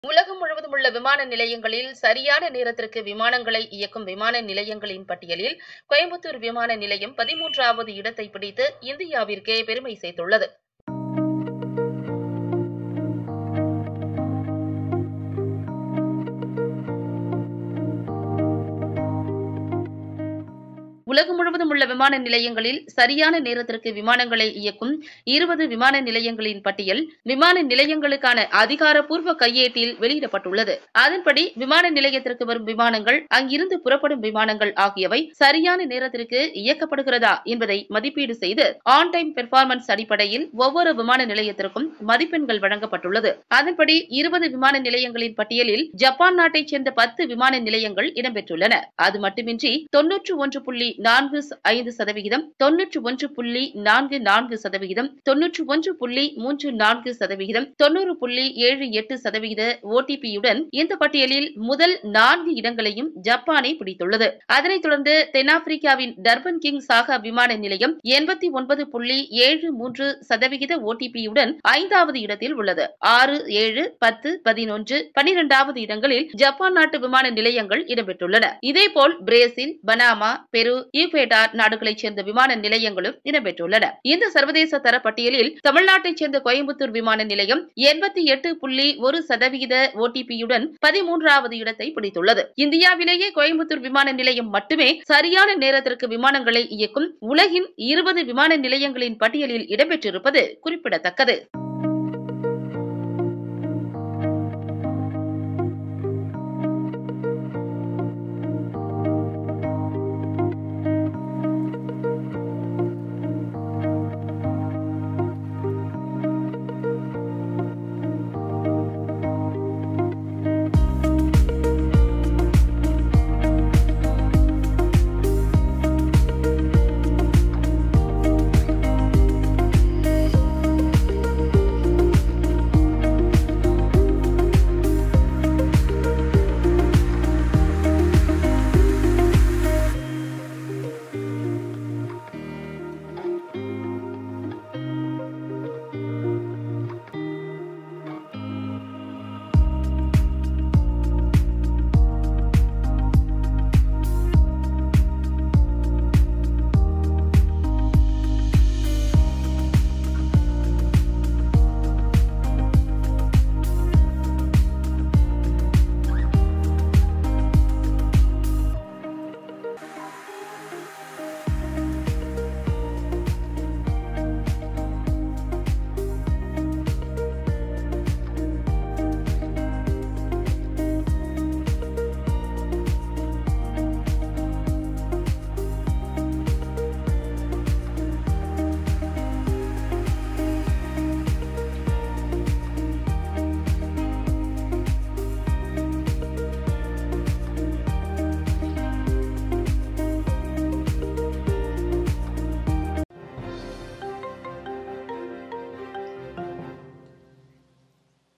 சரியான நேரத்திற்கு விமான சேவை - உலகில் டாப் 20 விமான நிலையங்களில் இடம்பிடித்த கோவை!
Coimbatore airport has ranked among top 20 international airports in the world for timely arrival and departure services